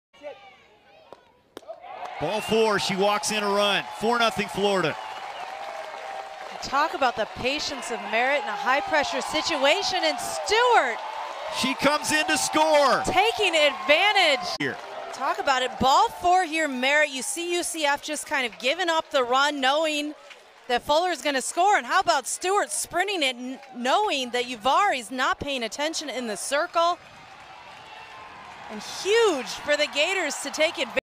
Remember When Florida Stole Home?
Throwing it back to when Florida stole home on a bases loaded walk! Get two for the price of one! Incredible base running by the Florida Gators.